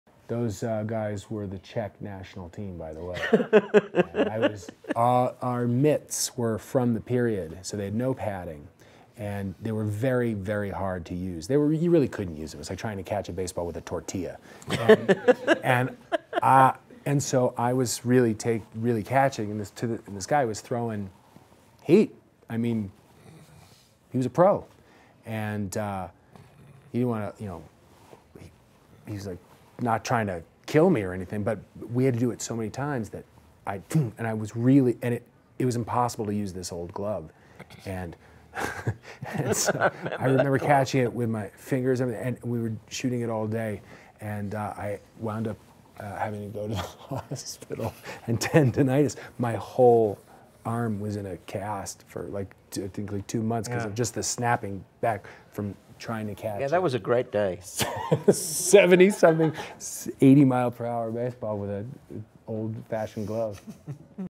Paul Rudd had to go to the hospital due to a baseball injury
Variety Studio presented by AT&T | Sundance 2018